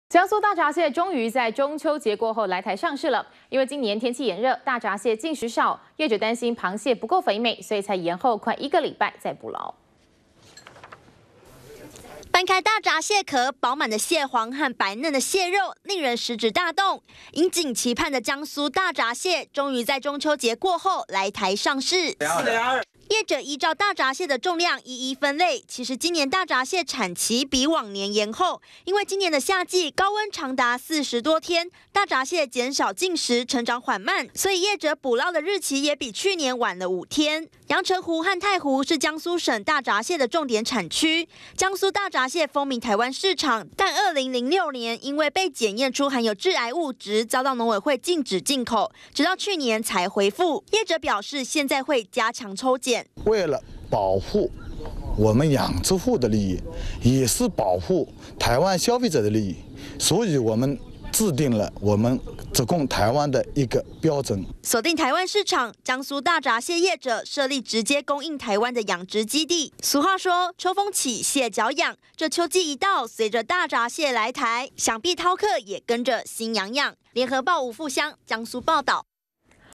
【2013.09.21】江蘇大閘蟹 秋節後來台誘饕客 -udn tv
江蘇大閘蟹終於在中秋節過後，來台上市了，由於今年天氣炎熱，大閘蟹進食少，業者擔心螃蟹不夠肥美，所以才延後快一個禮拜再捕撈。